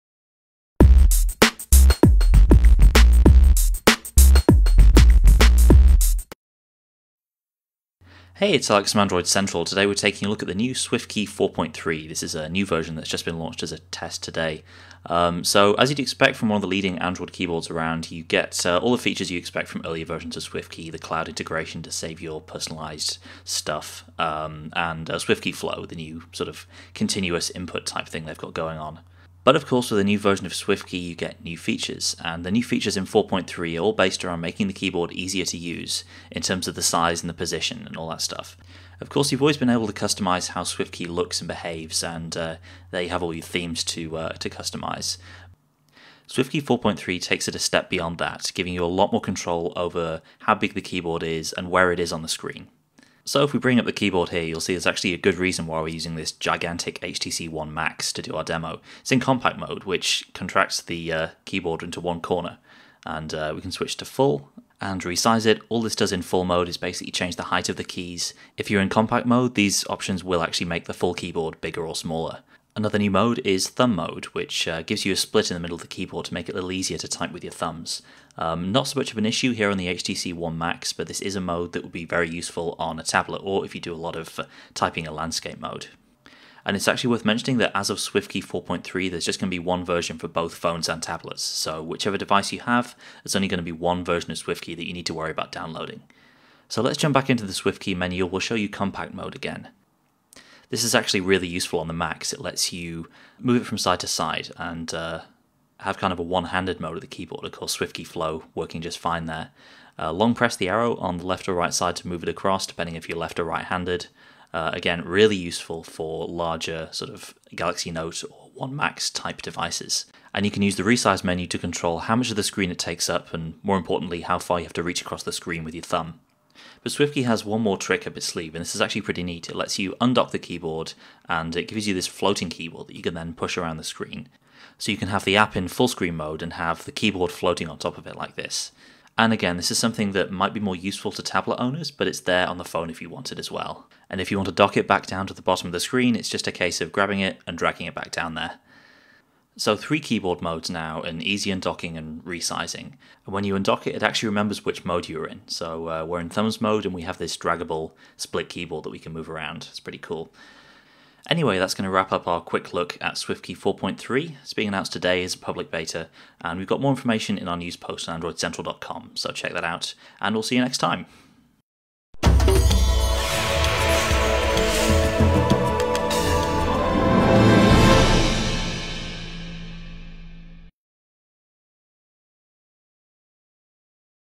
Hands-on with SwiftKey 4.3
New 'thumb,' 'compact' and undocked modes make it easier to type on larger screens